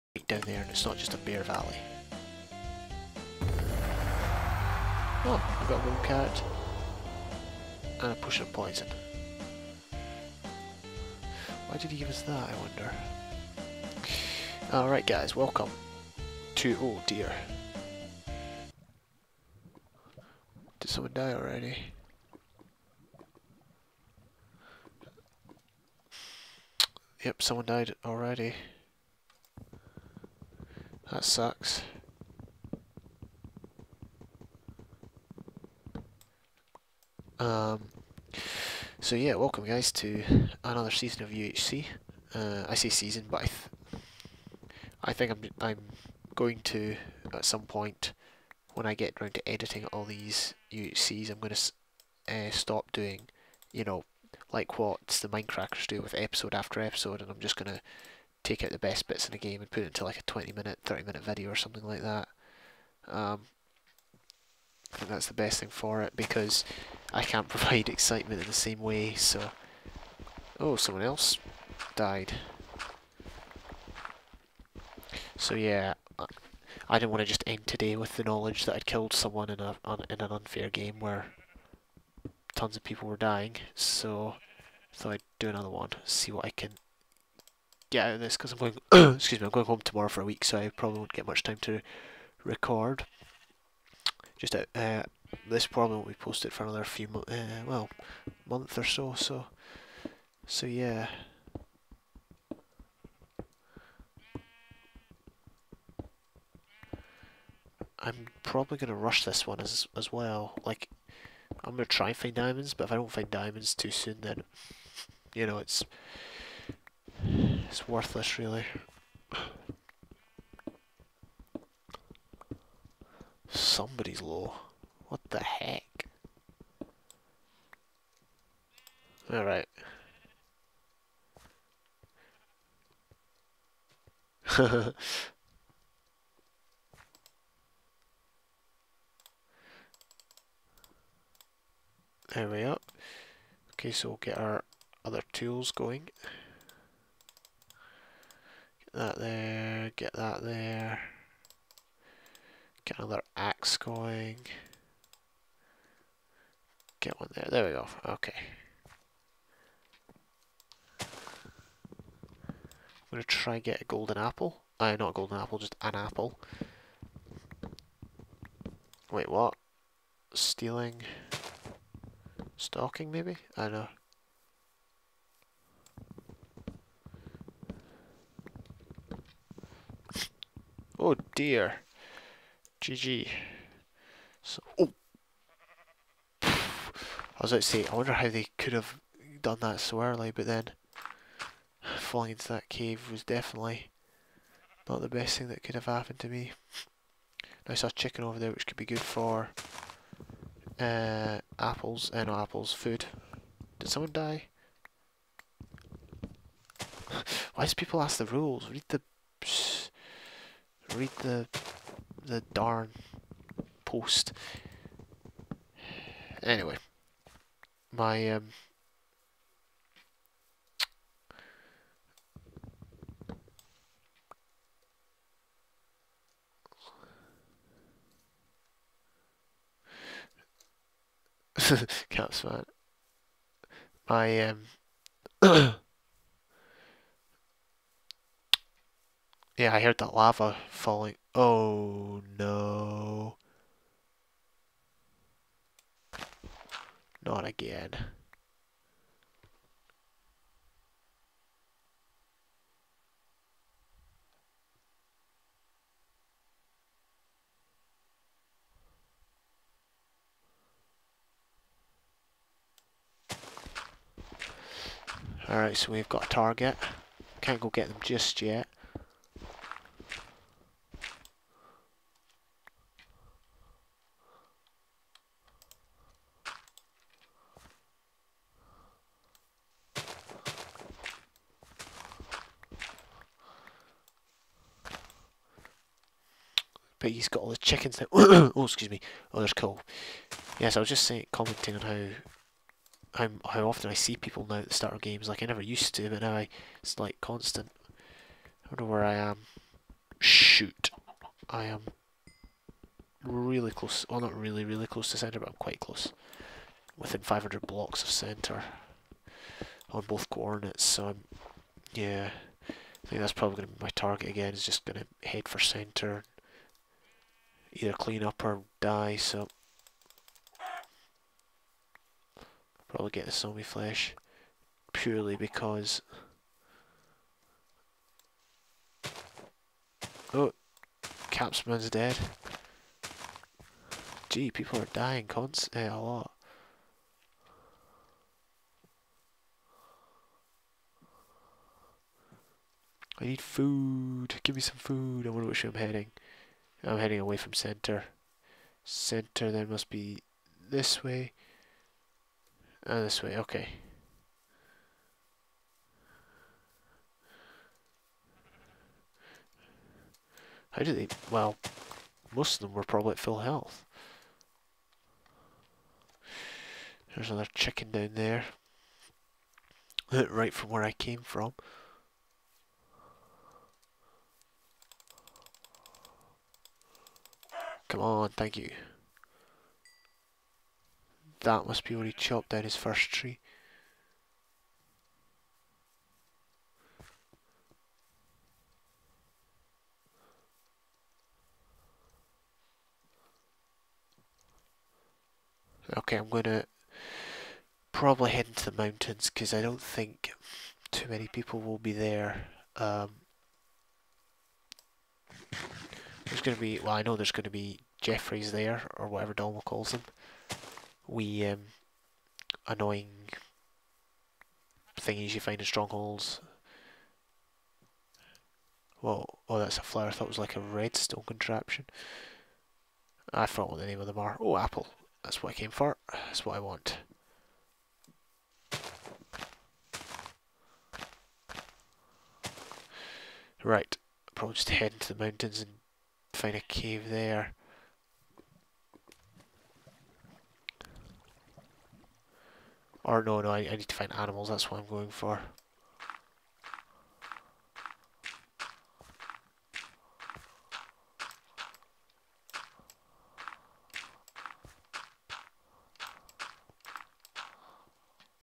Ultra Hardcore Game 25 - Episode 1
UltraHardcore is a PVP game in which health regeneration does not exist except through the use of potions of healing and golden apples. It is a last team/man standing game through survival in PVE or PVP.